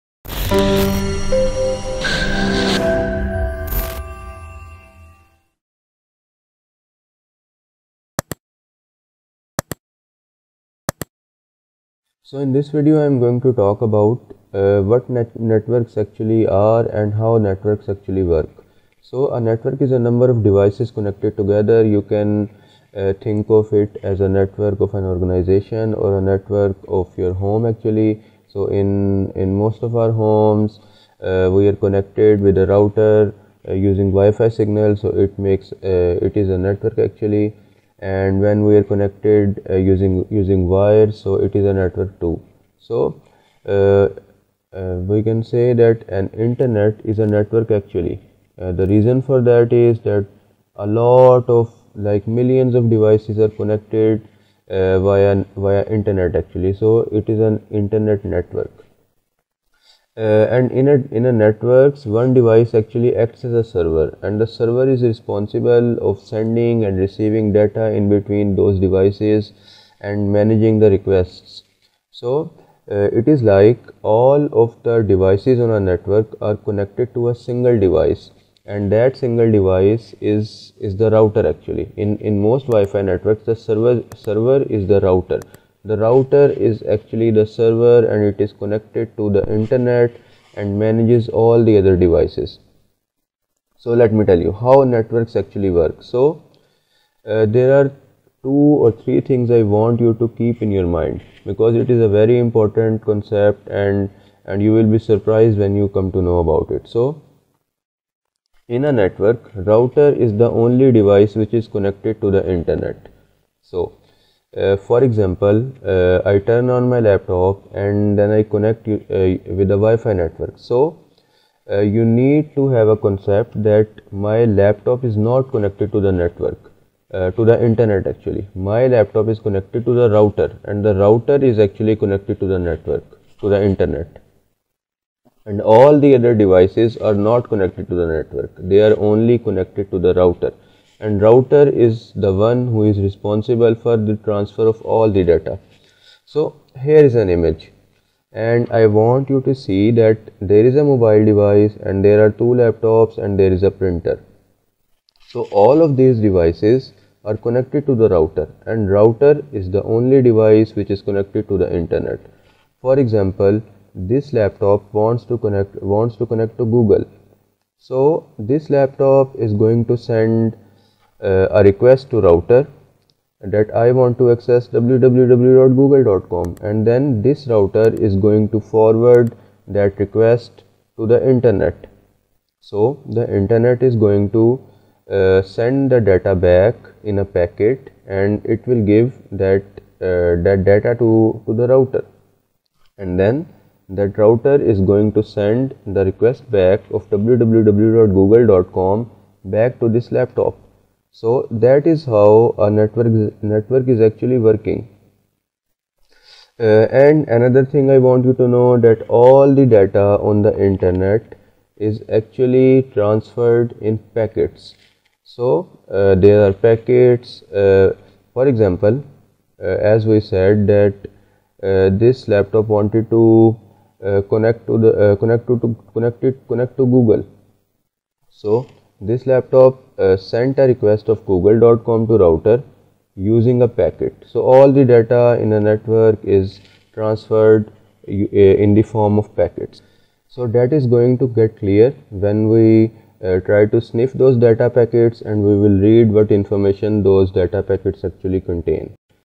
13. How Networks Actually Work - How Internet Is Provided To Devices In A Network
In this video, you are going to learn how router provides internet to all the devices connected to it, how data packets are sent in a network and how router forwards requests to all those devices connected to it.
Please join our Facebook group if you have any questions related to the course or other IT queries.